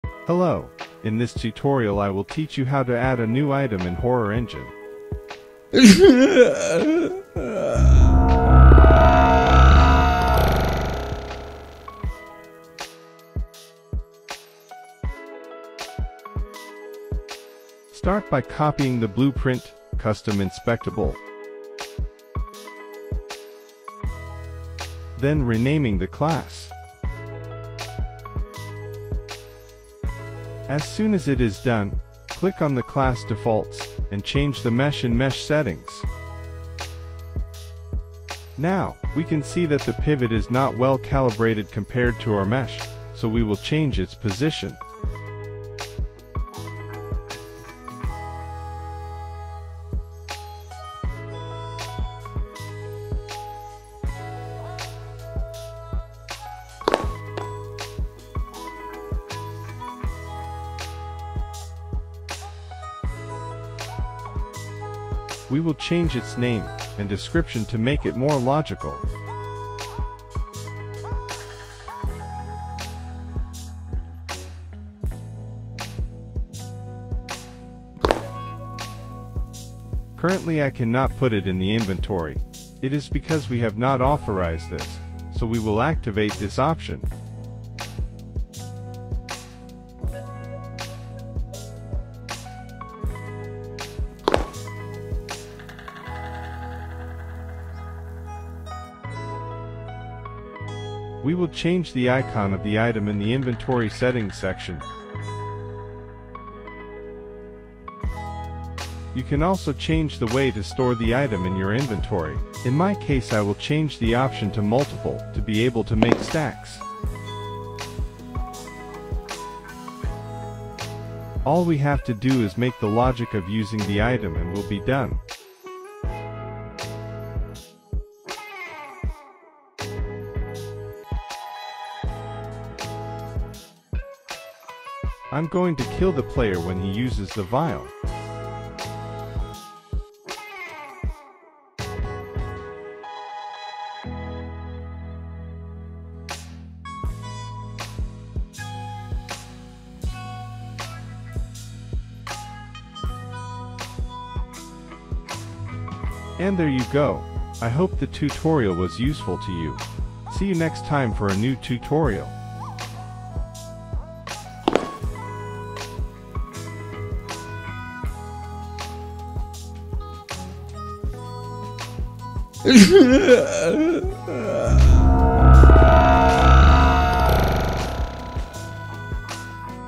[UE5] Horror Engine - Create Custom Items #10
Hello, in this tutorial I will teach you how to add a new item in Horror Engine. For my part the item that I will create will be a vial of poison.
Start by copying the Blueprint "Custom Inspectable" then renaming the class.
As soon as it is done, click on the Class Defaults and change the mesh in Mesh Settings.
Now, we can see that the pivot is not well calibrated compared to our mesh, so we will change its position.
Let's see in our scene what it looks like for the moment.
So we have a functional item.
We will change its name and description to make it more logical.
Currently I can not put it in the inventory, it is because we have not authorized this, so we will activate this option.
We will change the icon of the item in the Inventory Settings section, you can also change the way to store the item in your inventory, in my case I will change the option to multiple to be able to make stacks.
All we have to do is make the logic of using the item and we'll be done, I'm going to kill the player when he uses the vial.

Intro - 00:00
Create the BP - 00:13
Change mesh and pivot - 00:32
Set name and description - 01:04
Set item as takeable - 01:24
Set icon and storage type - 01:40
Create the use logic - 02:06
Outro - 02:43